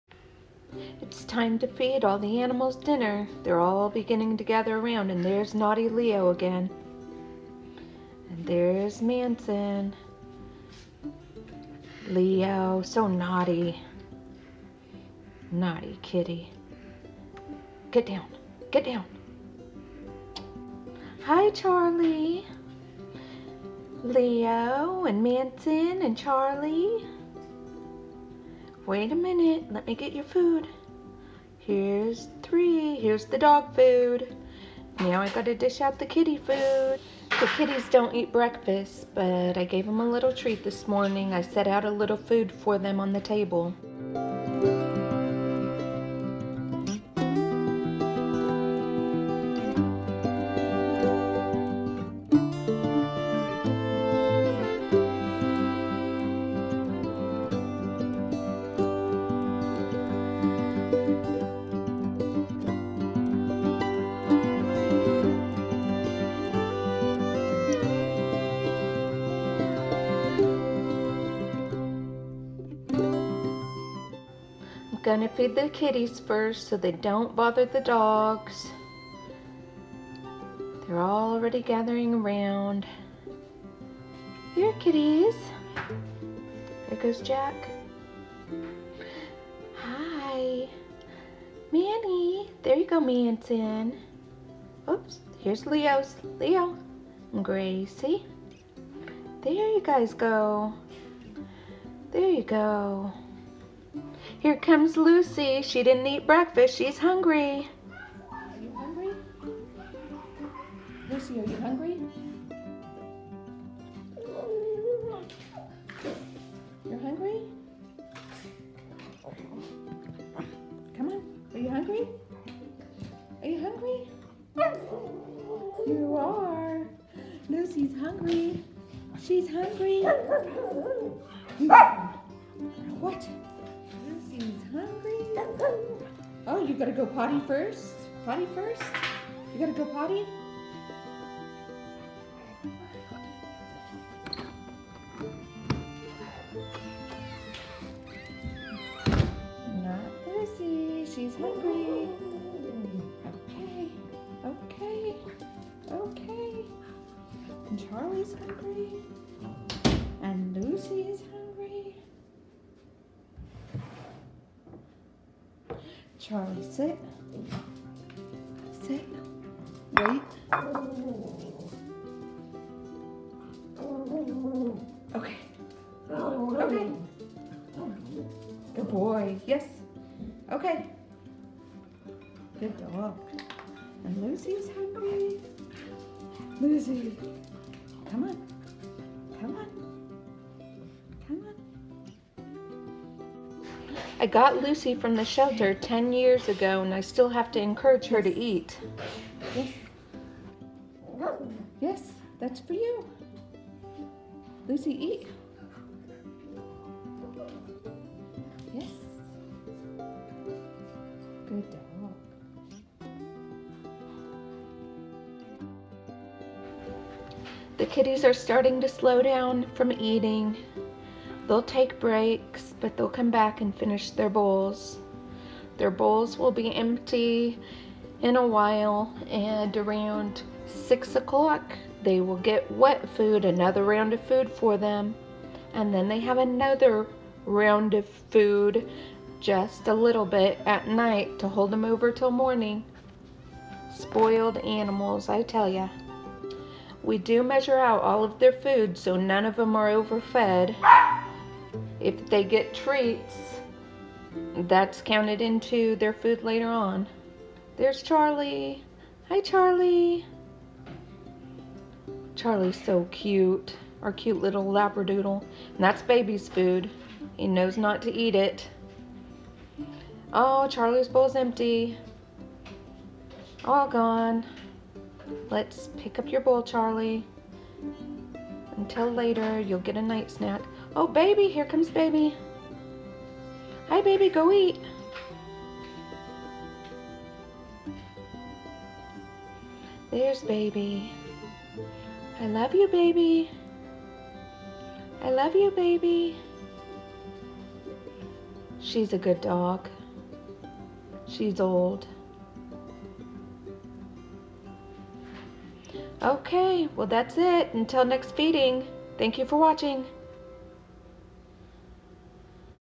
Feeding cute dogs and cute kitties
Join me as I feed our six animals
Leo the naughty yellow cat
Manson the beautiful blue
Gracie calm old cat

Lucy our old red dog.
Baby our big black dog
Charlie are miniature Labradoodle doodle